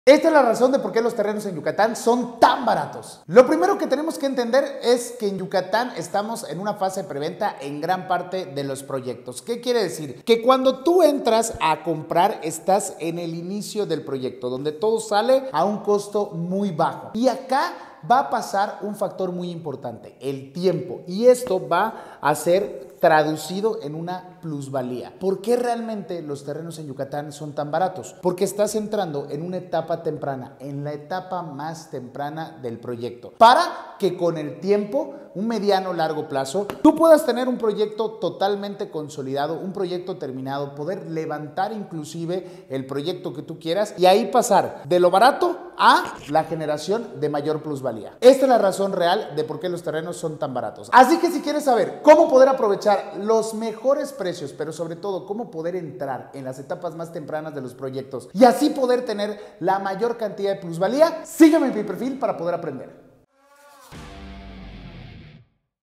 ESTA ES LA RAZÓN DE PORQUE EN YUCATÁN LOS TERRENOS SON MUY BARATOS
Hoy te platico porque en Yucatán actualmente los terrenos son muy baratos y fácil de poder invertir en ellos.